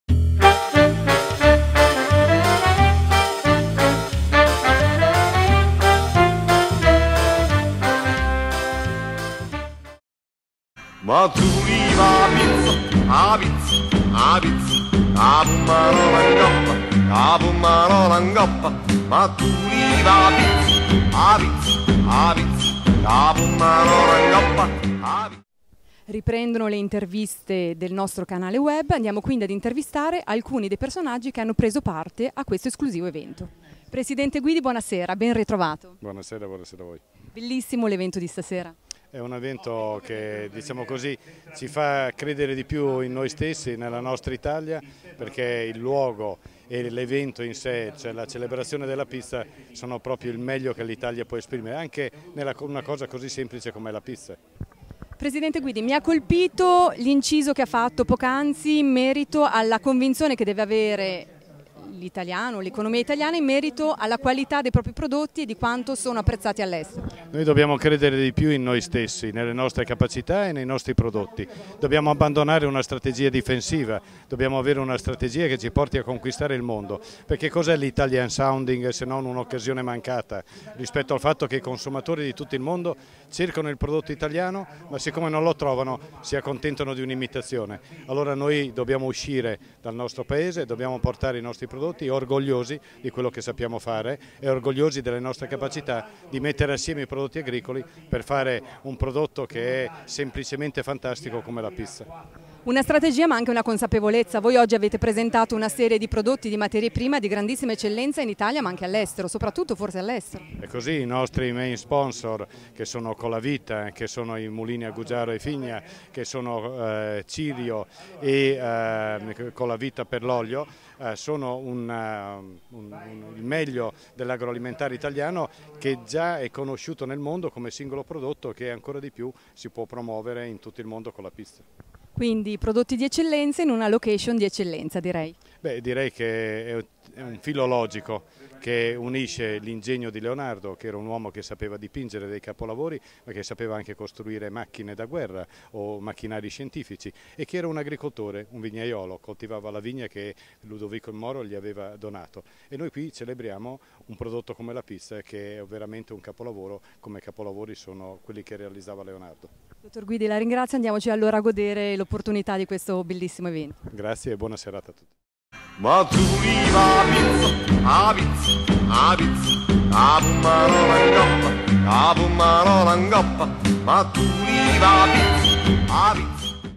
CONFAGRICOLTURA: La Pizza Capolavoro Universale - Intervista a Mario Guidi
Intervista a  Mario Guidi, Presidente di CONFAGRICOLTURA